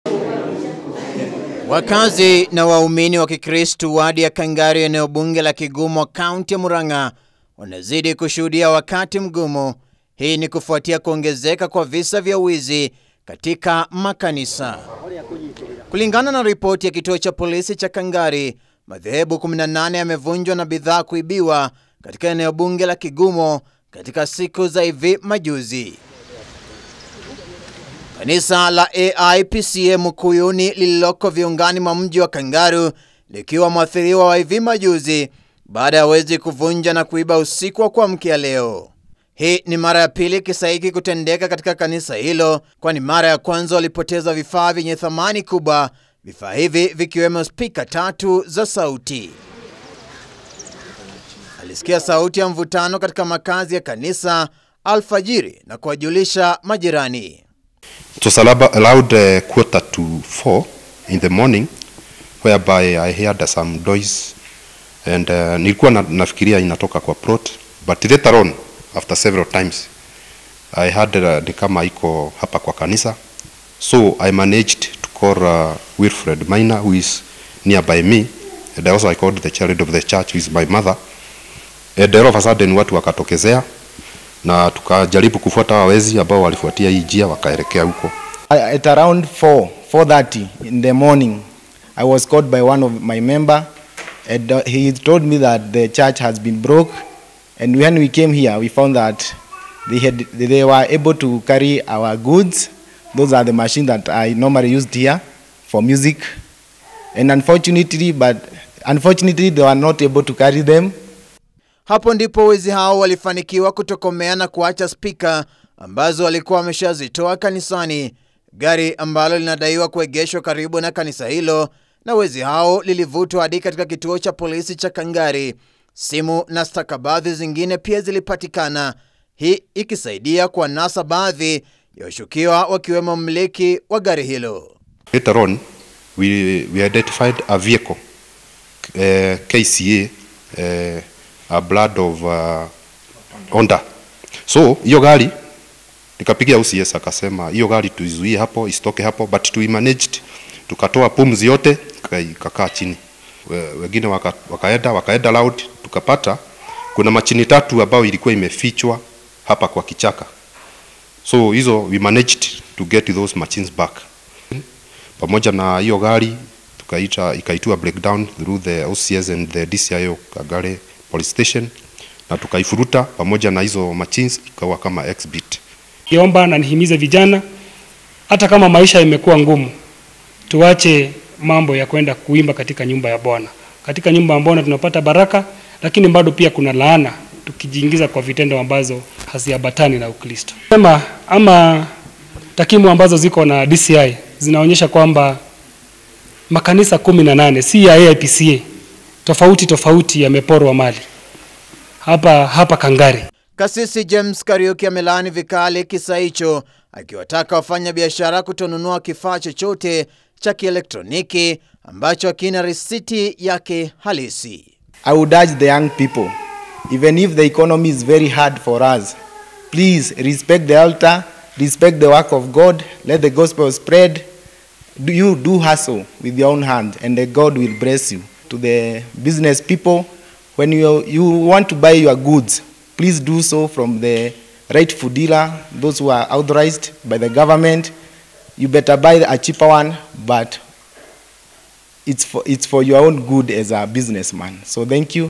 Visa Vya Wizi Kanisani Kaunti Ya Murang'a Vyaongezeka
Visa Vya Wizi Katika Makanisa Katika Wadi Ya Kangari Eneo Bunge La Kigumo Kaunti Ya Murang'a Vimeongezeka.Kulingana Na Ripoti Ya Kituo Cha Polisi Cha Kangari,Kufikia Sasa Makanisa 18 Yamevunjwa Eneo Hilo Na Bidhaa Zake Kuibia,Kisa Cha Hivi Majuzi Kikiwa Katika Kanisa La Mukuyu-Ini Mjini Kangari Usiku Wa Kuamkia Leo.